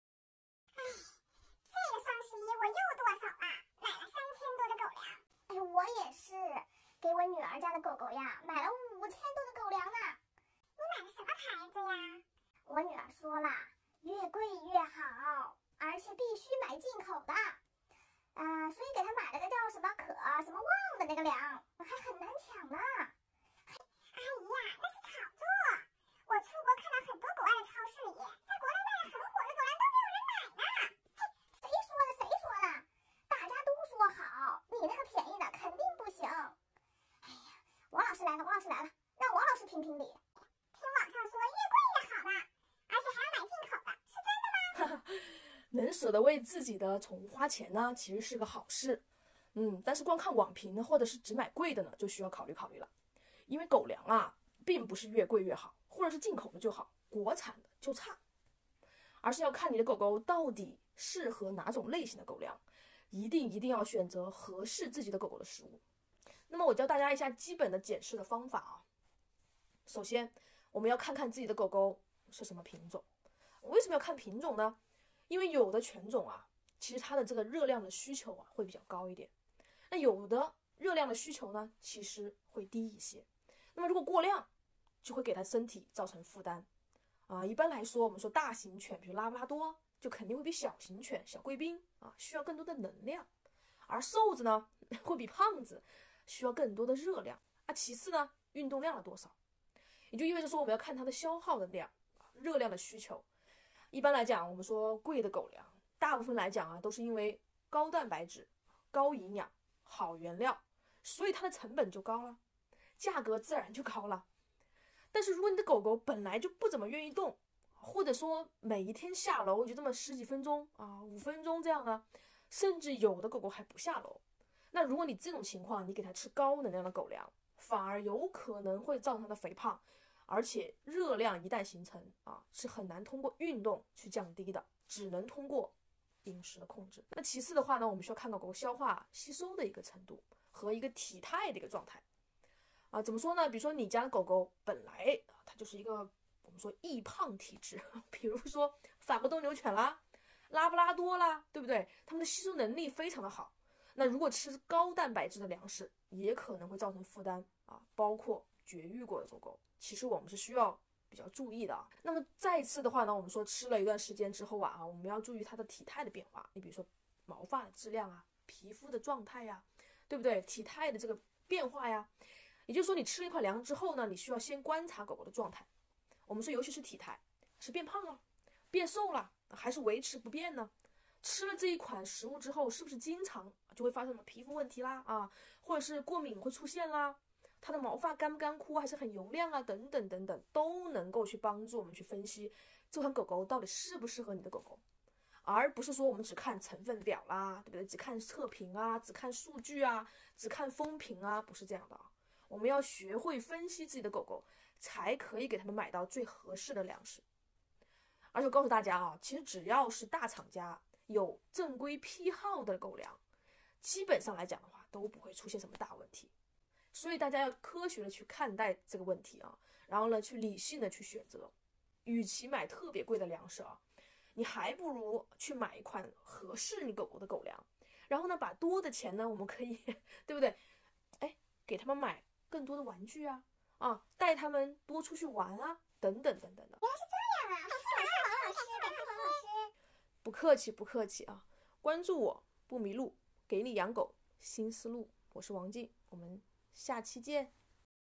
狗粮真的越贵越好么？！双十二别再瞎买了！
很贵的进口粮真的有必要么？！双十二别再瞎买狗粮了！现在宠物经济的崛起，各种牌子如雨后春笋般的出来，特别都家长都不知道如何挑选，总是选择贵的不停的看测评购买，但是贵的测评的真的就是适合我们自己狗狗的吗？今天我们一起来学习科学选择最适合你家狗狗的一款粮食！